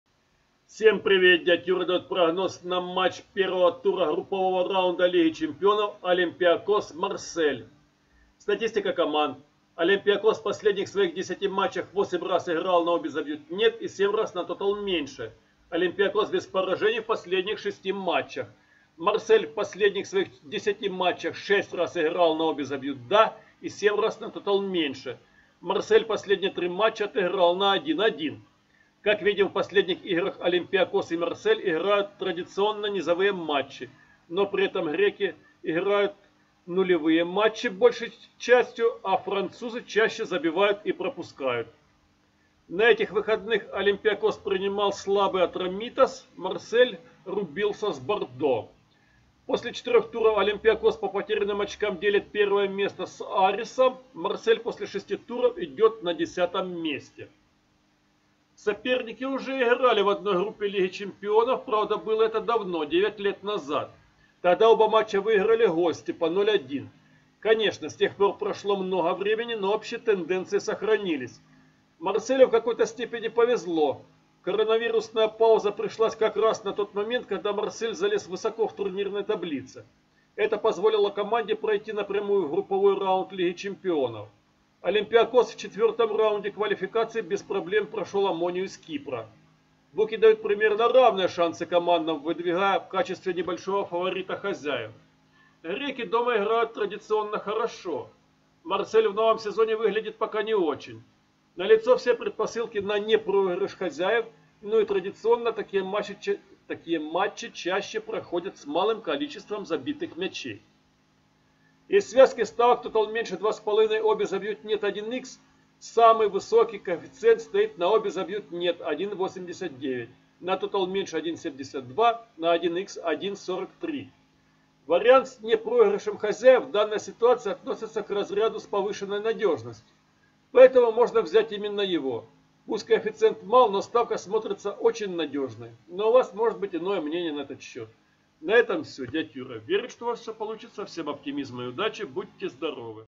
Олимпиакос - Марсель, прогноз 21 октября (лига чемпионов, 1 тур)
Прогноз на матч Олимпиакос - Марсель (21.10), статистика и варианты ставок